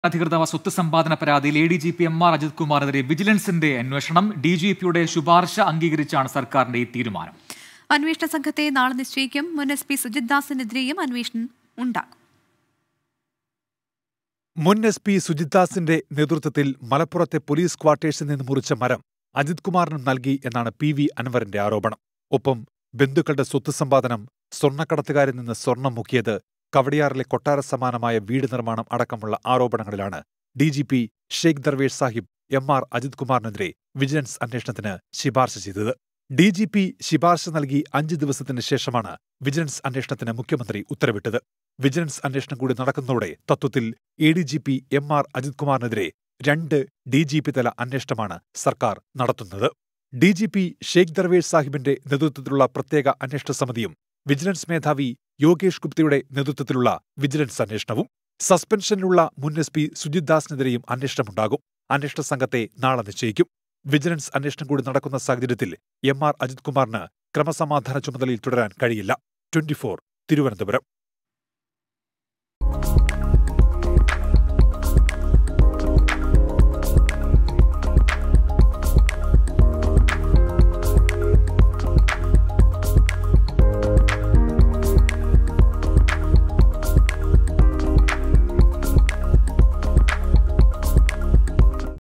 ADGP എംആർ അജിത് കുമാറിനെതിരെ വിജിലൻസ് അന്വേഷണം; അന്വേഷണ സംഘത്തെ നാളെ നിശ്ചയിക്കും
Vigilance inquiry against ADGP MR Ajith Kumar; The investigation team will be appointed tomorrow